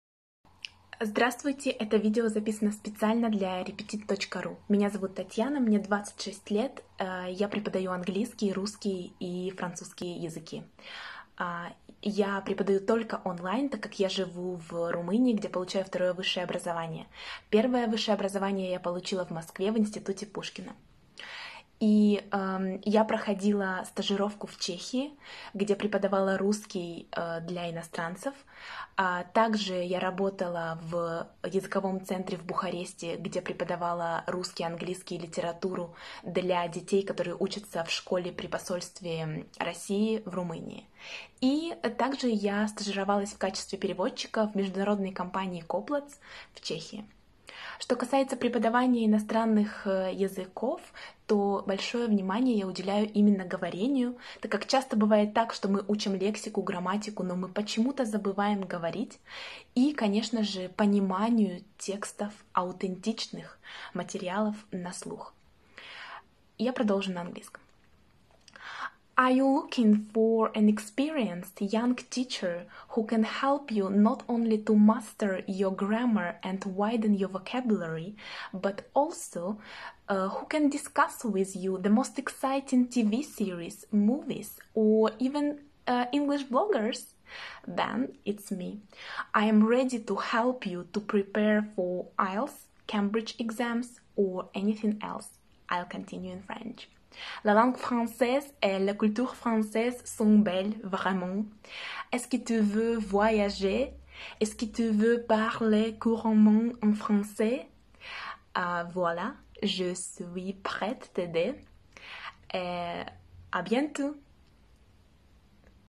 Билиневич Татьяна Александровна - репетитор по английскому языку - видеопрезентация для Repetit.ru
Видеопрезентация репетитора по английскому, французскому и русскому языкам и литературе Билиневич Татьяны Александровны для REPETIT.RU.

Билиневич Татьяна Александровна окончила Государственный институт русского языка им А.С. Пушкина.